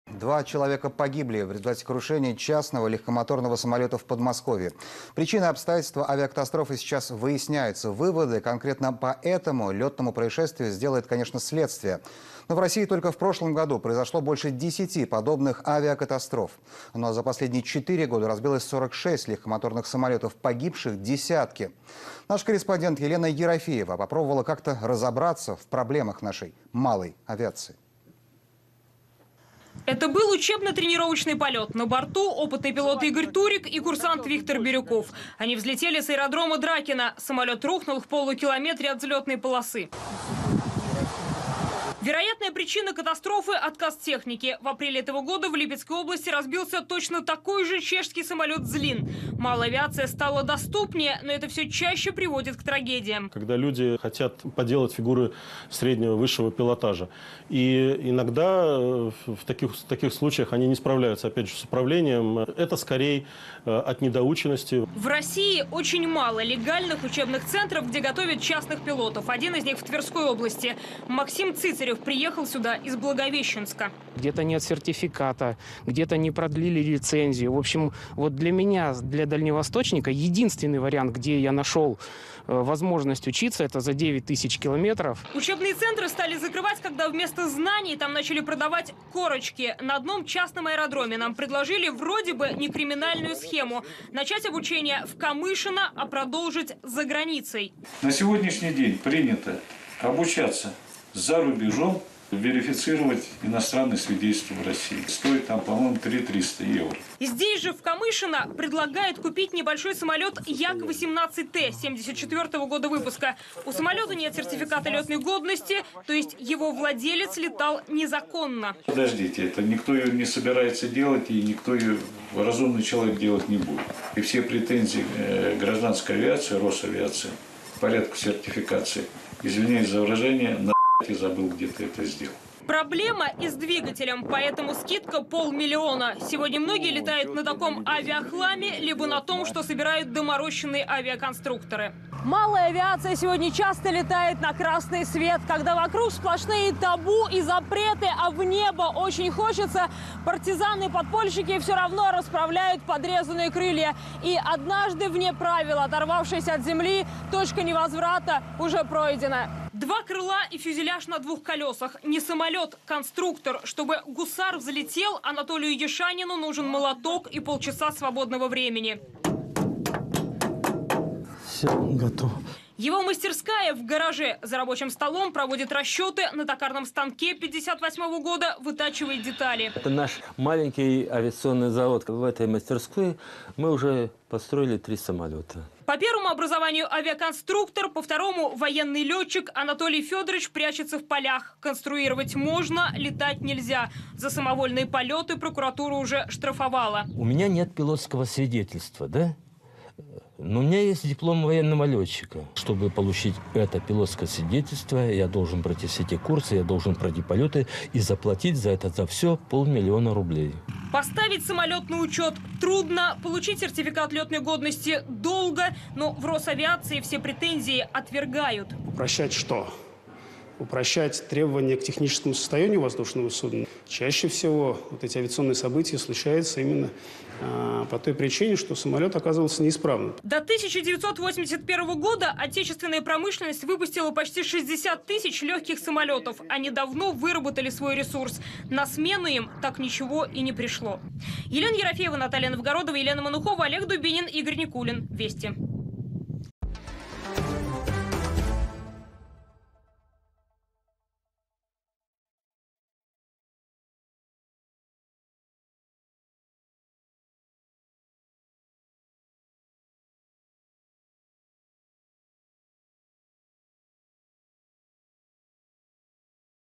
Авиакатастрофа в Подмосковье Почему российские частные самолеты падают все чаще Россия 24 YouTu
Два человека погибли в результате крушения частного легкомоторного самолета в Подмосковье. Причины и обстоятельства авиакатастрофы выясняются. Выводы по этому летному происшествию сделает следствие. Но в России только в прошлом году произошло более 10 подобных катастроф.